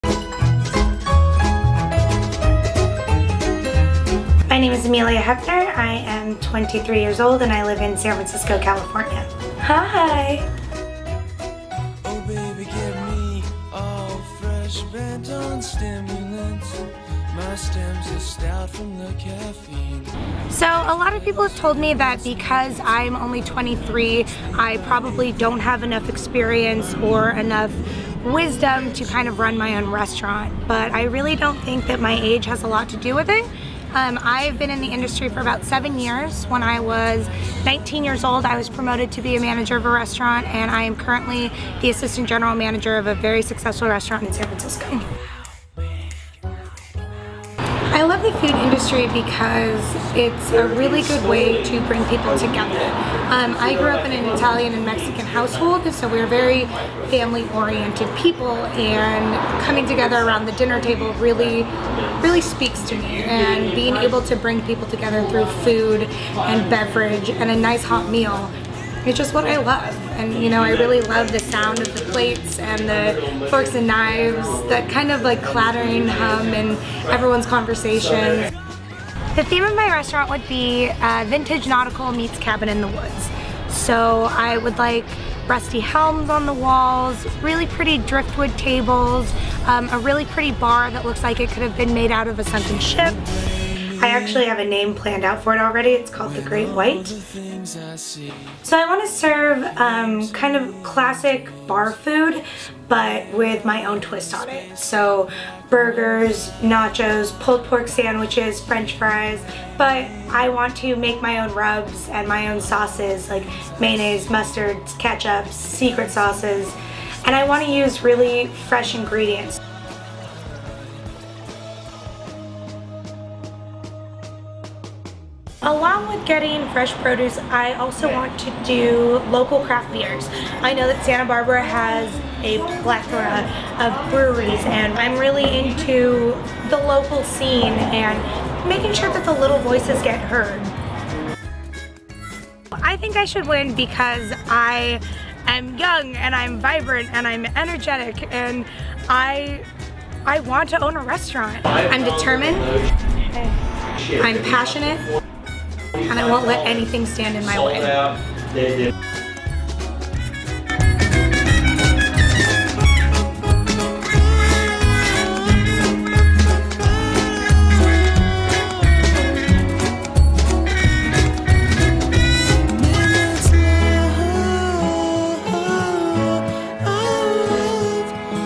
Amelia Heppner - Pace Restaurant Contest video
Help me win!

My name is Amelia Heppner and I want this so badly! I've been in the food industry for 7 years and its been my dream to own my own restaurant !

Like this video please!